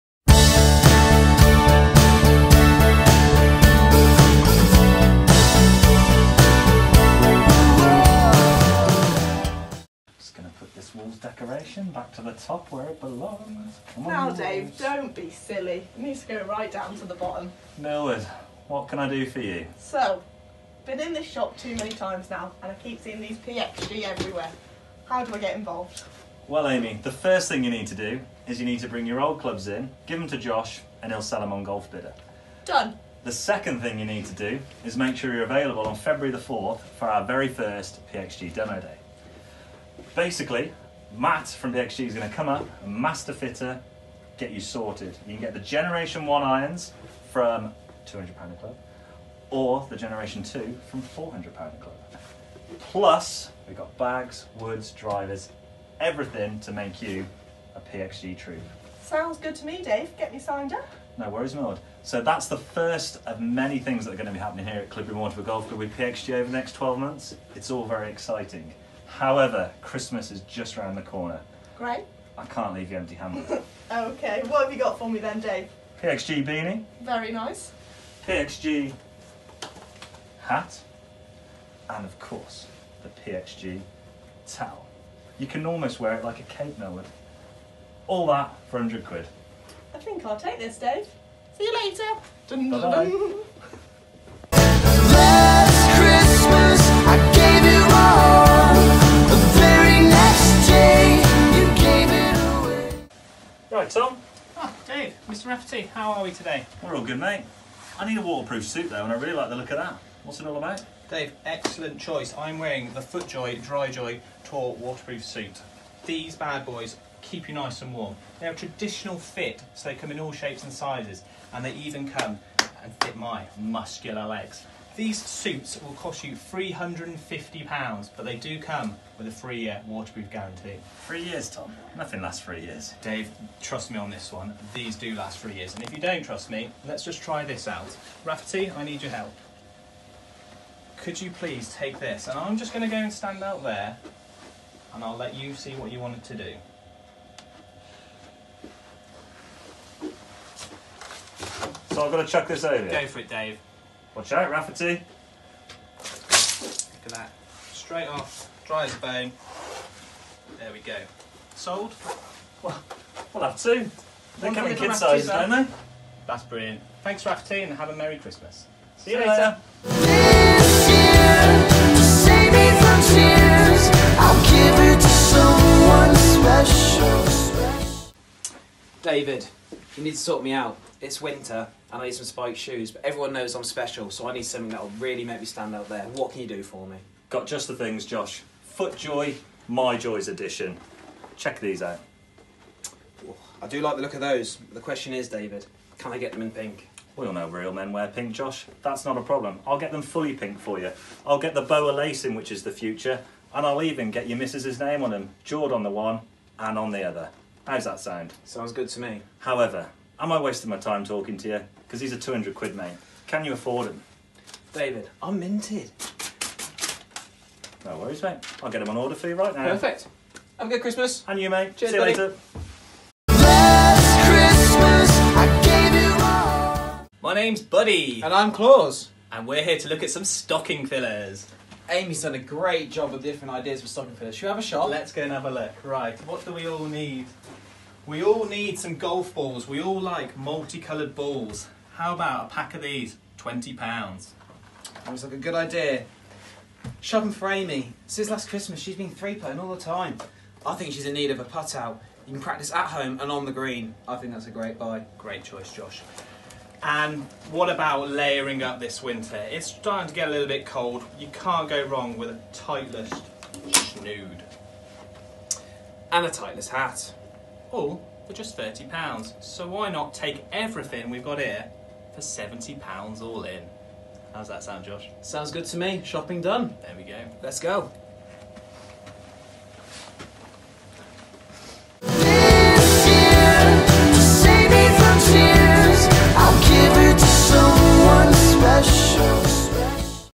Christmas Video 2018
The team at Cleobury Mortimer Golf Club are back once again for their annual Christmas Video. All for a bit of fun and a little laugh.